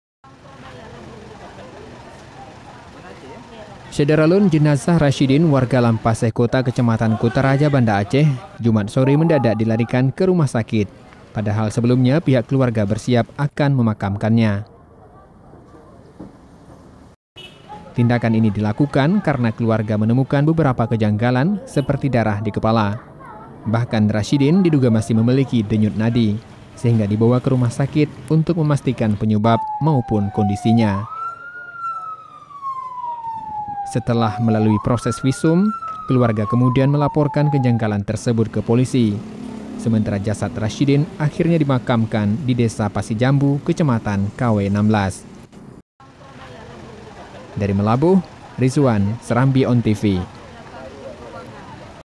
Sempat Batal Dimakamkan, Jasad Pensiunan Guru di Aceh Barat Dilarikan ke Rumah Sakit
SERAMBITV.COM, MEULABOH - Jenazah Rasyidin (62) warga Lampaseh Kota Kecamatan Kutaraja, Banda Aceh, Jumat (25/1/2019) sore mendadak dilarikan ke rumah sakit, padahal sebelumnya pihak keluarga bersiap akan memakamkannya.

Jasad almarhum yang merupakan pensiunan guru itu dievakuasi ke Rumah Sakit Cut Nyak Dhien Meulaboh, Aceh Barat untuk dilakukan visum.

Tindakan ini dilakukan karena keluarga menemukan beberapa kejanggalan seperti darah di kepala.

Bahkan, Rasyidin diduga masih memiliki denyut nadi, sehingga dibawa ke rumah sakit untuk memastikan penyebab maupun kondisinya.

Setelah melalui proses visum, keluarga kemudian melaporkan kejanggalan tersebut ke polisi, sementara jasad Rasyidin akhirnya dimakamkan di Desa Pasi Jambu Kecamatan Kaway XVI.